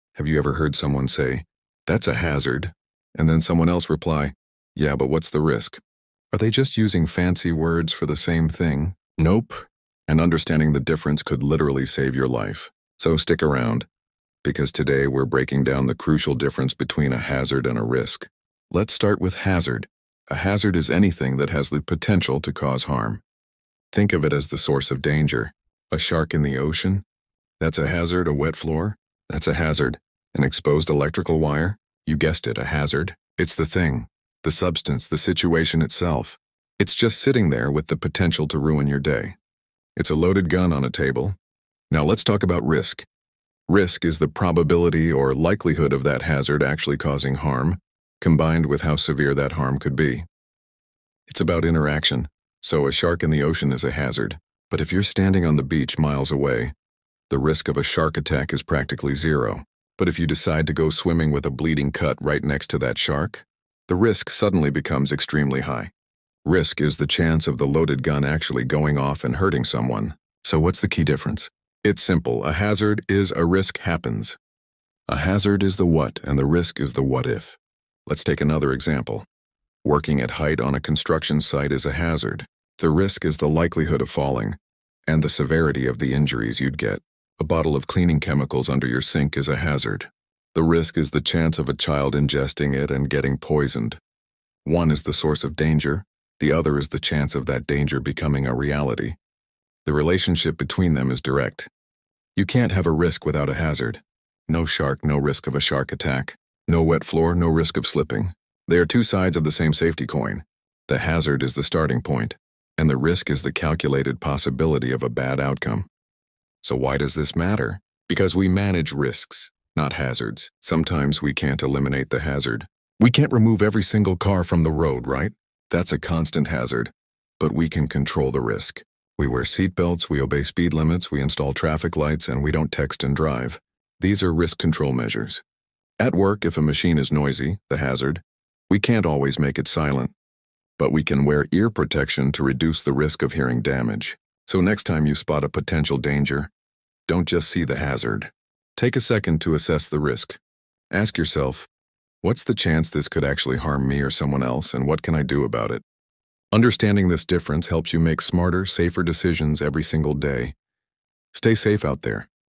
Hazard vs Risk: Key Differences, Definitions, and Workplace Safety
Understand the difference between hazard and risk with clear definitions, examples, and safety insights. Learn why identifying hazards and controlling risks is essential for workplace safety, accident prevention, and effective risk management.